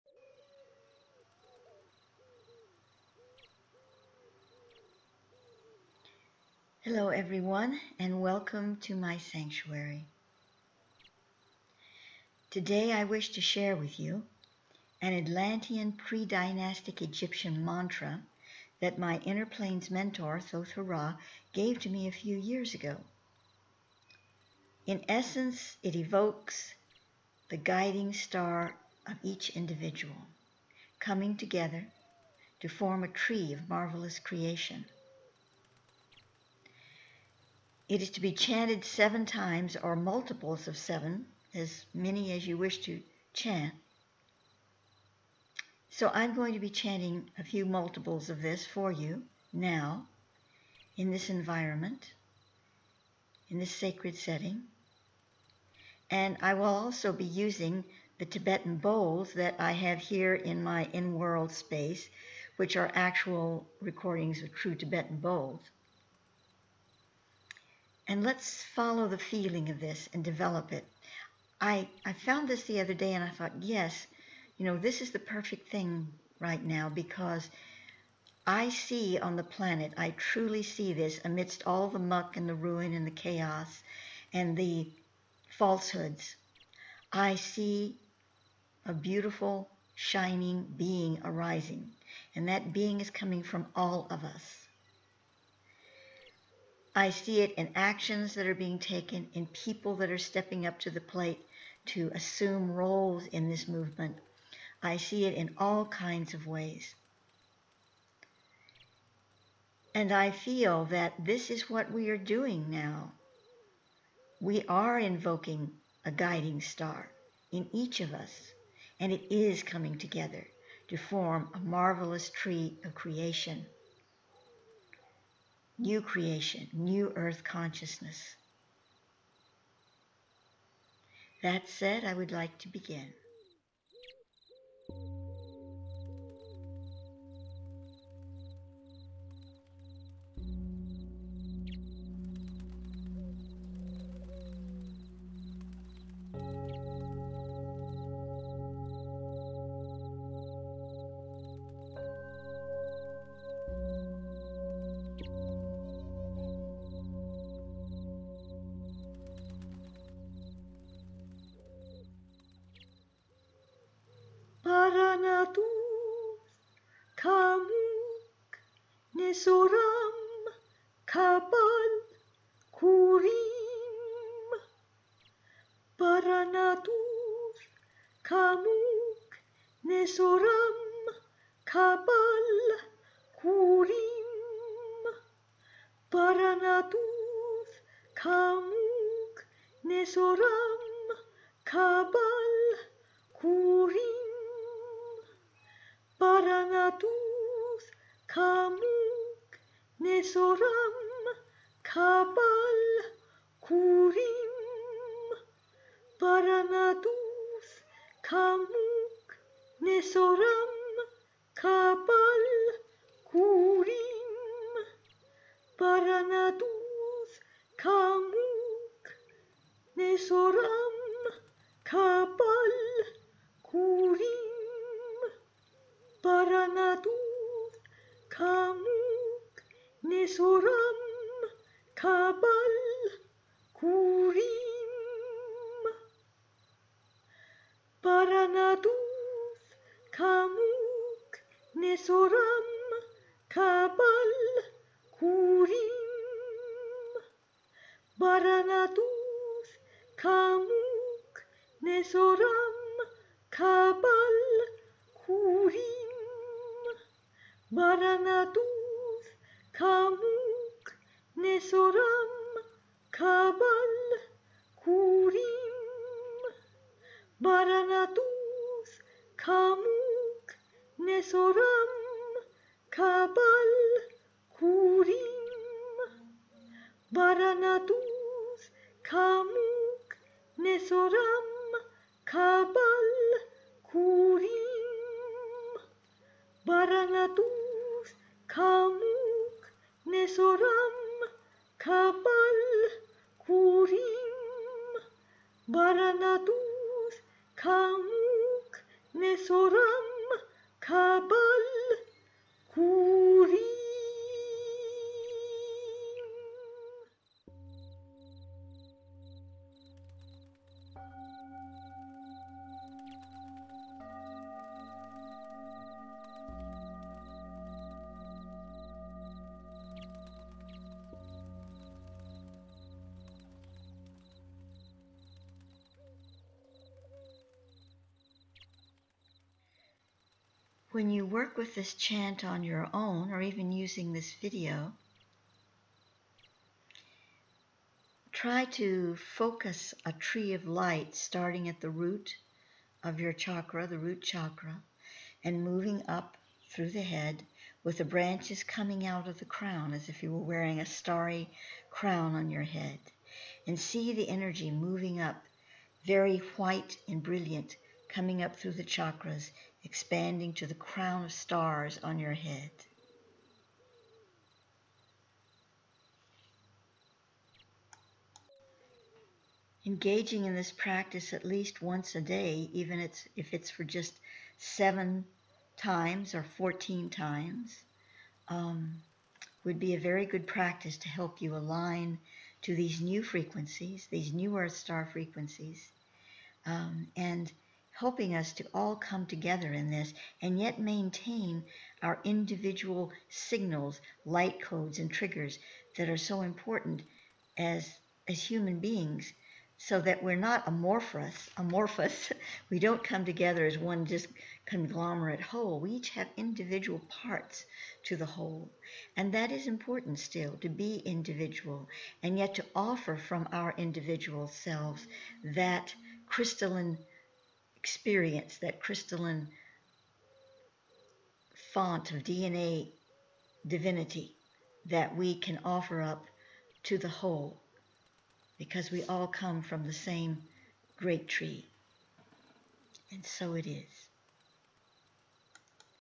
Marvelous Tree Mantra
The Mantra: BARANATUTH, KAMUK NESSORAM, KABAL KUREHM